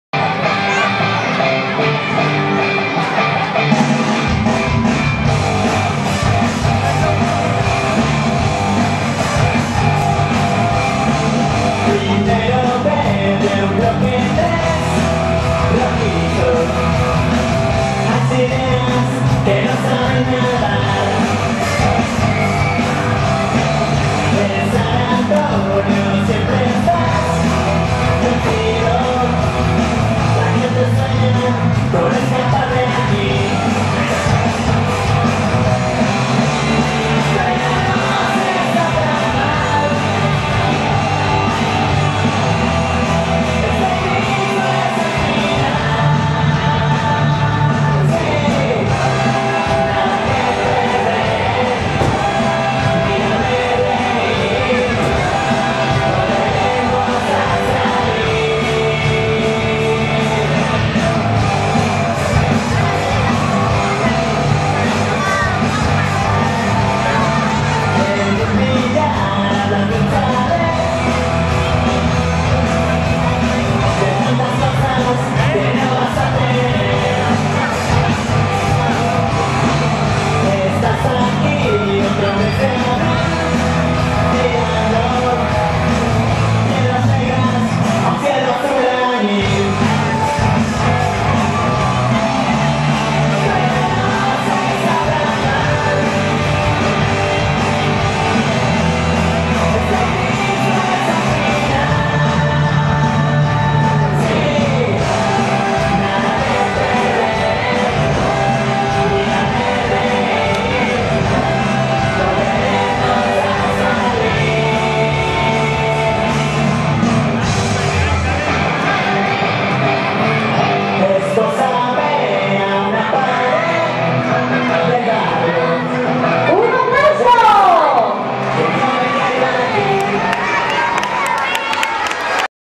Las Policias
Carnavales de Tarazona 2009 Baile y danza de disfrazados  a concurso